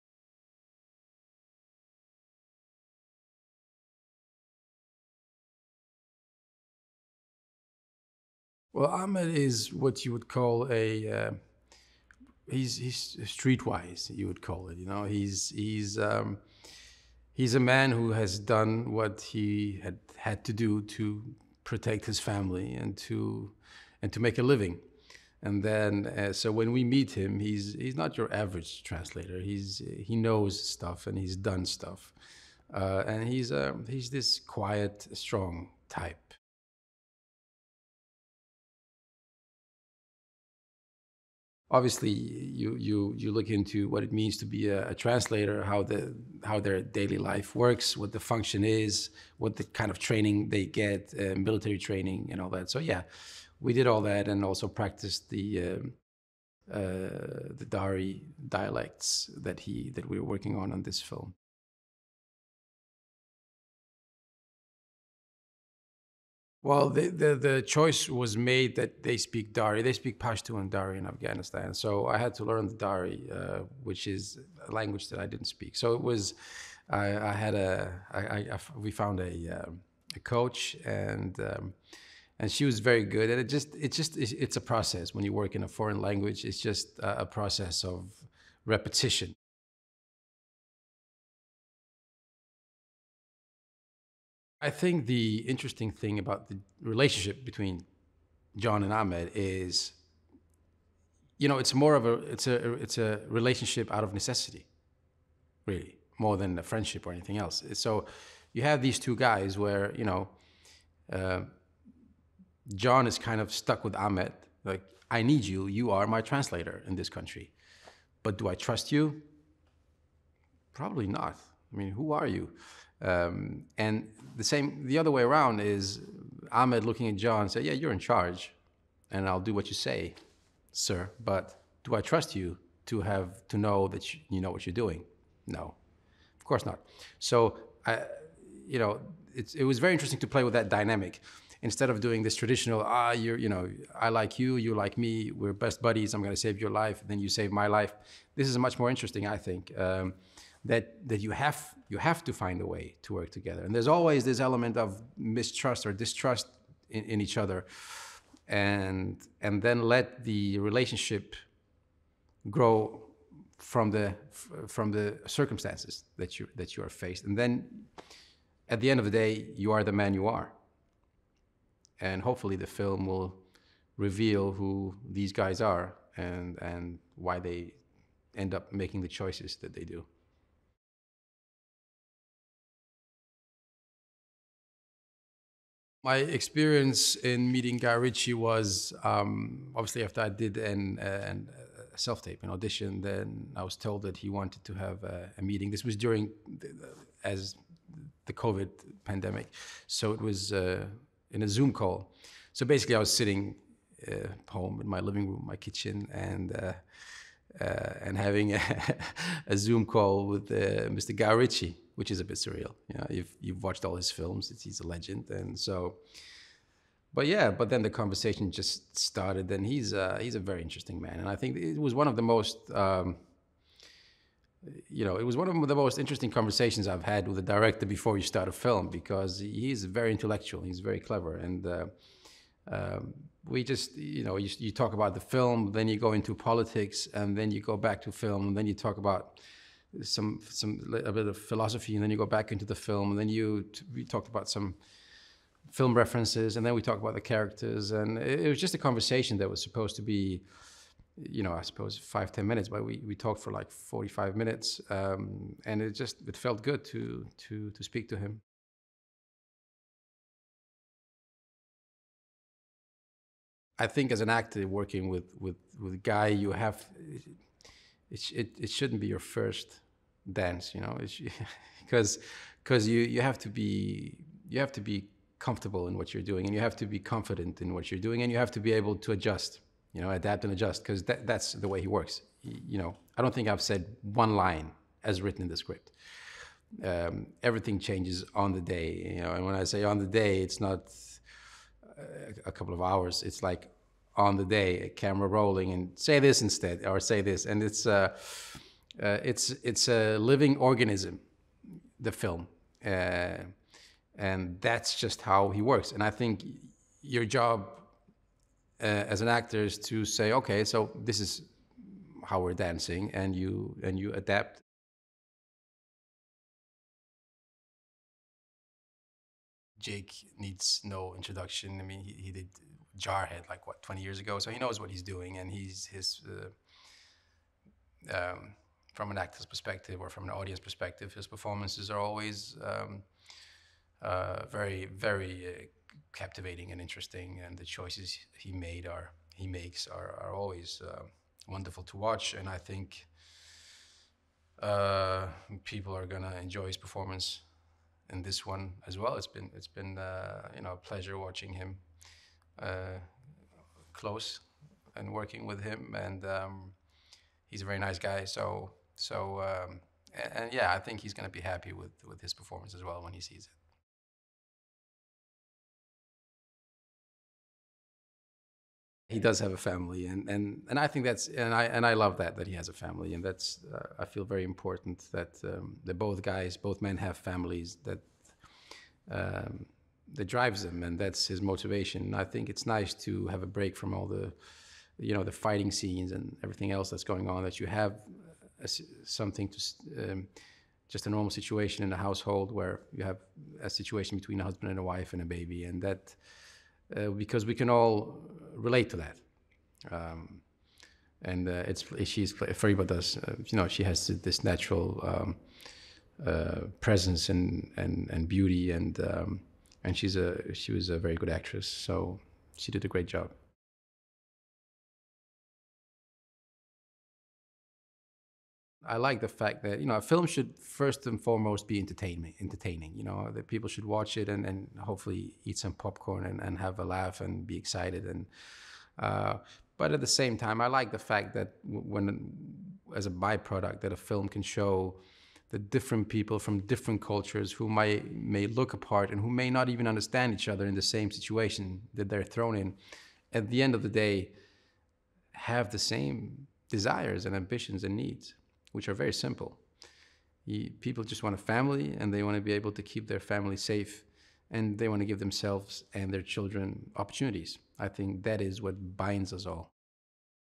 Guy Ritchie’s The Covenant - Dan Schmidt - Military Advisor | Interview
During the war in Afghanistan, a local interpreter risks his own life to carry an injured sergeant across miles of grueling terrain.